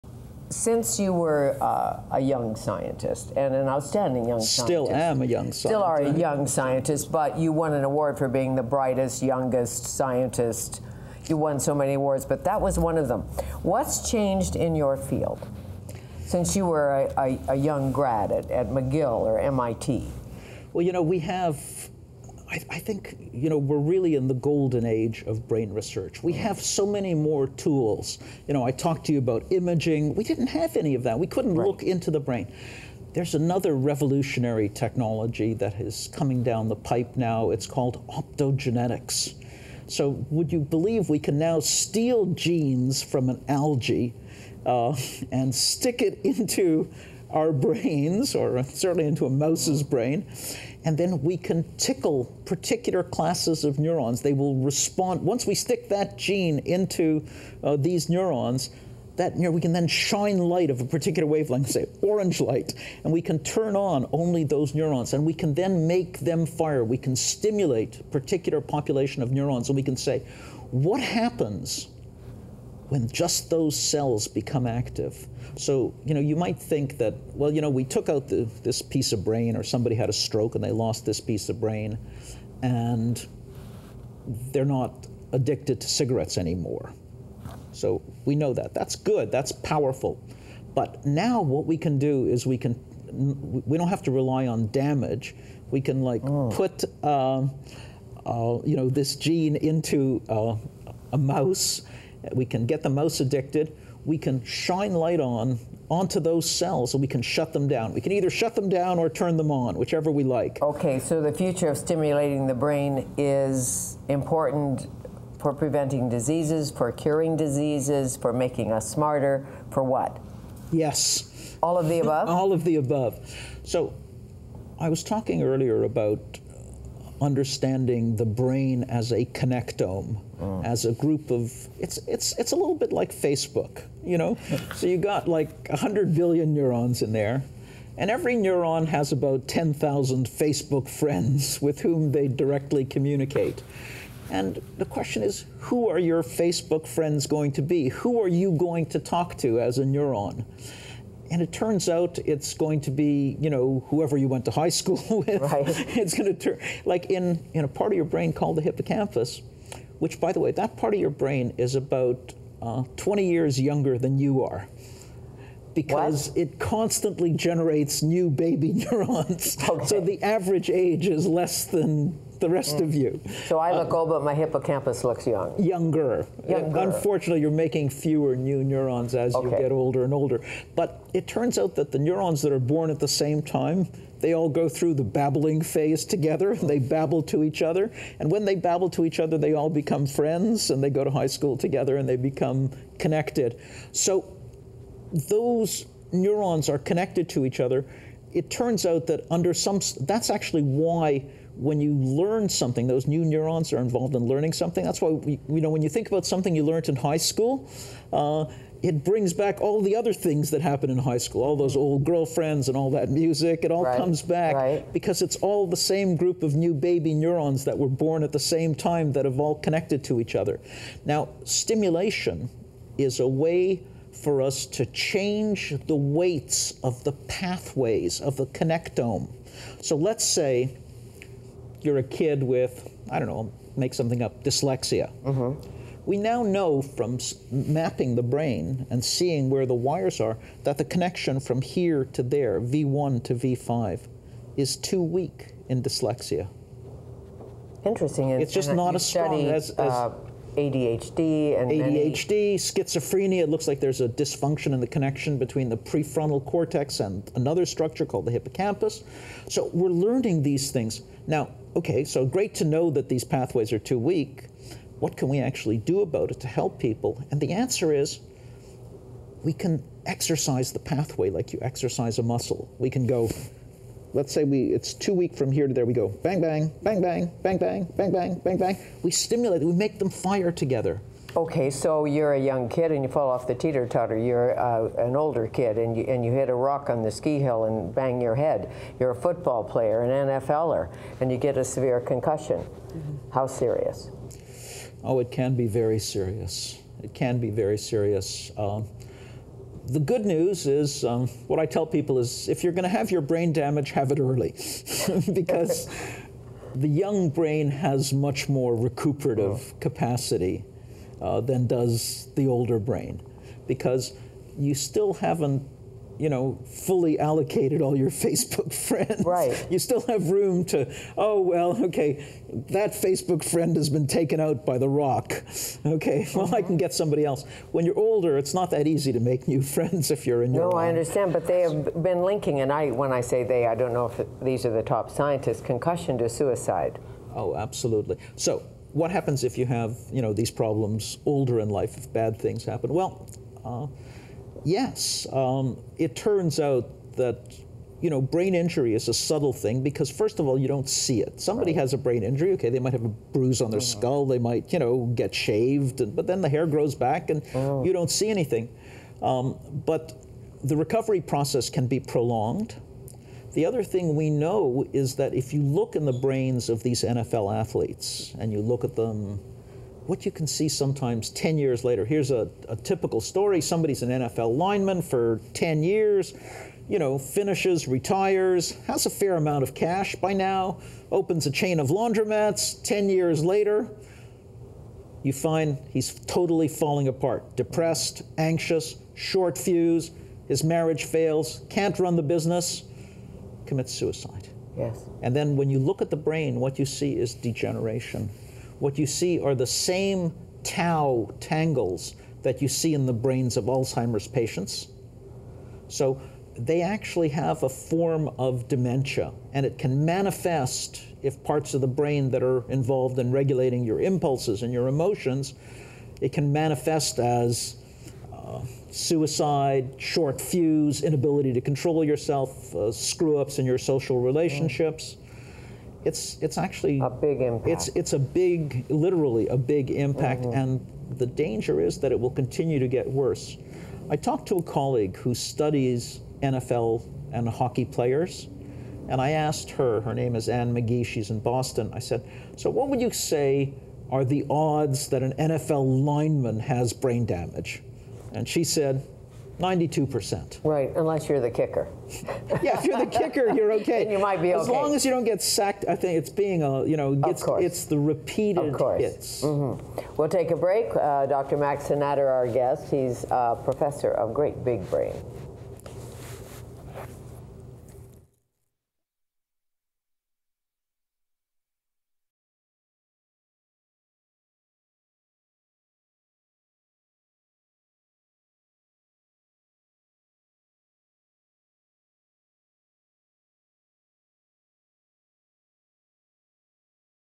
Director, The Brain Research Centre - Dr. Max Cynader - Part 2 of 3
Director of the Brain Research Centre and the Djavad Mowafaghian Centre for Brain Health at Vancouver Coastal Health and UBC, Dr. Max Cynader, speaks with Fanny Kiefer about brain connectivity and the next big breakthrough in 10 years